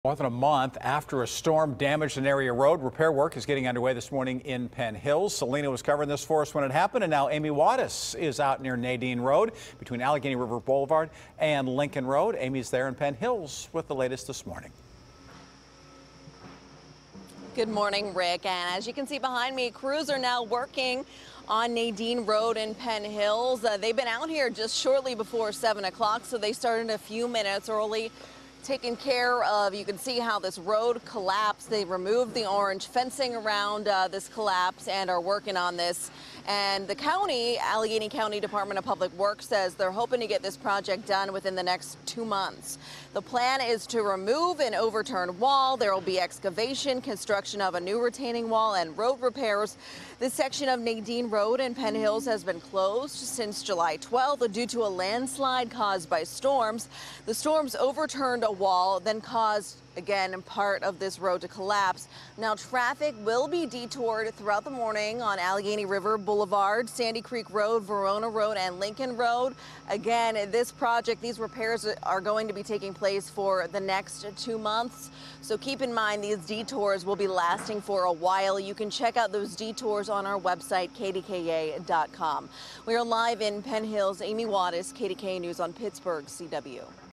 Crews Beginning Repairs On Storm-Damaged Road In Penn Hills
Repairs are underway this morning on a storm-damaged road in Penn Hills.